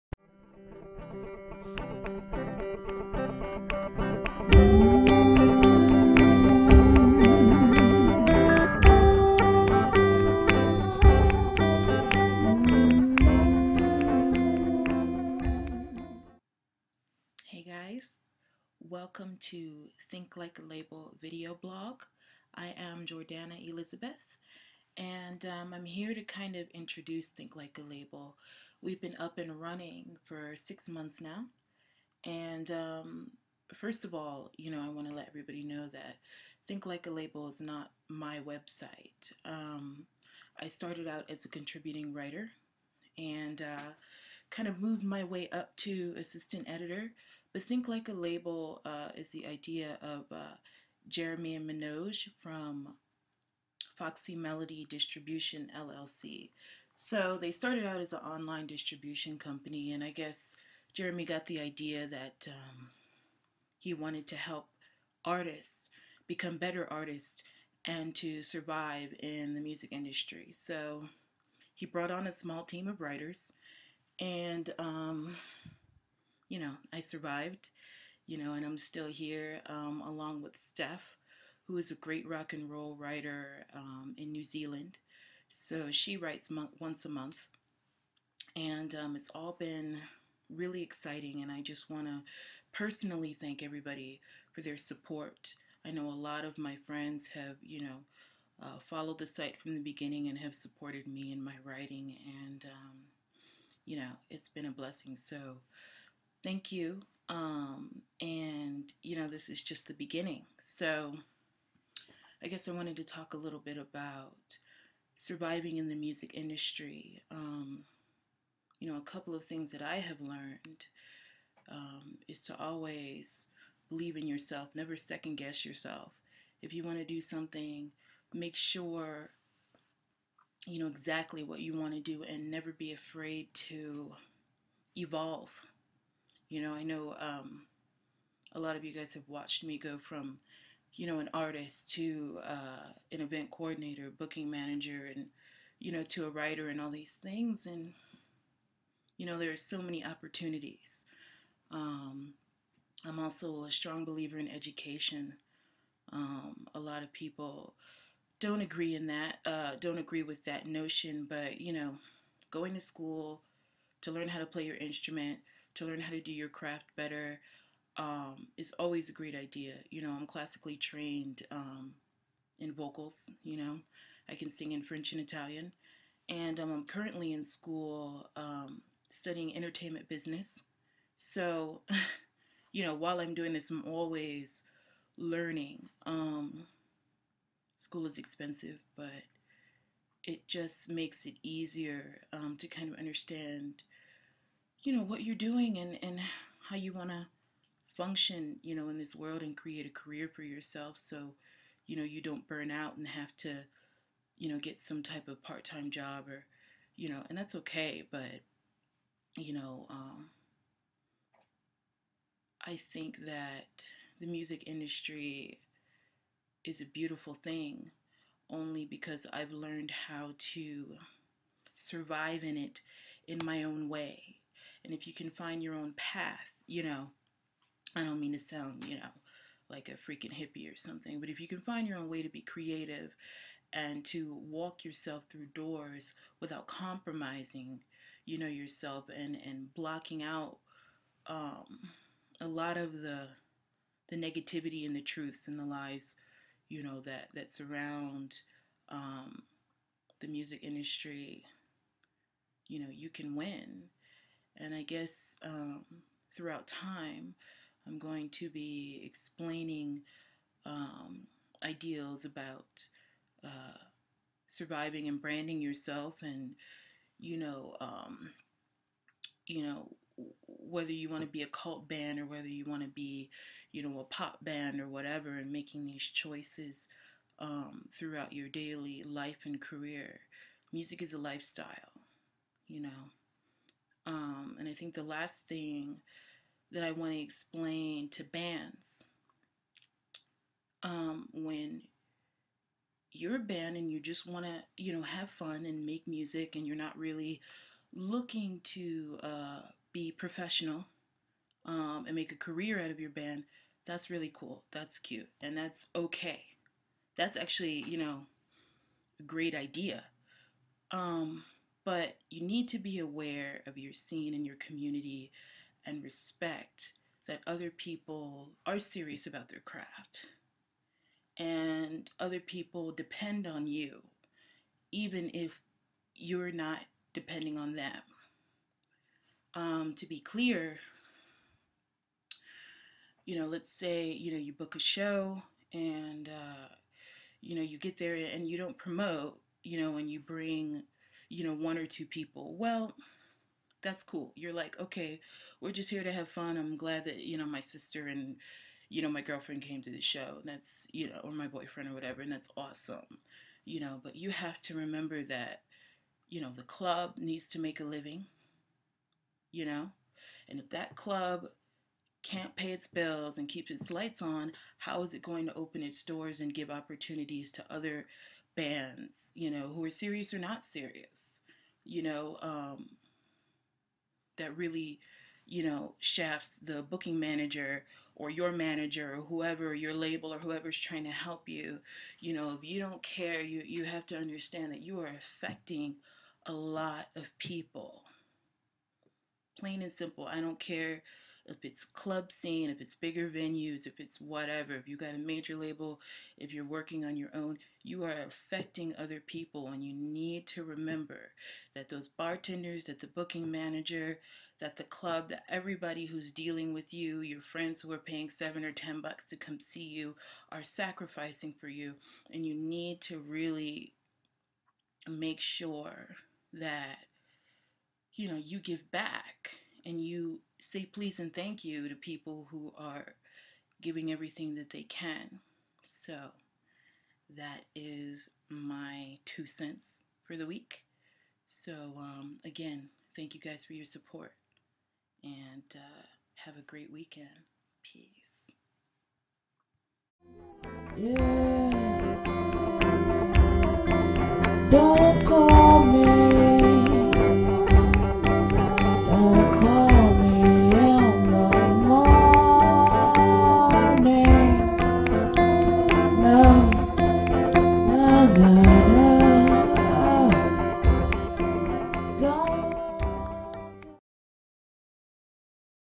Think Like a Label Video Blog Episode 1
It was really fun to goof off with Ricky Reed of Wallpaper. a couple of weeks ago, but the music industry isn't all about meeting and interviewing big producers and musicians.

That doesn't happen for a very long time. There are come initial steps to take when starting out as a band and I'm interested in talking about a couple of those topics.

This is Think Like A Label's first official video blog that will be hosted by me, and other musicians and industry professionals.

Today, I took the time to introduce myself and the site and to thank our readers for being so amazing and supportive. We can't make it without you. If you have any questions or comments, feel free to write to us or comment on our individual posts. Me and the rest of the staff are available to openly answer your questions.

Thanks!  You're awesome!

Jordannah and Think Like A Label Team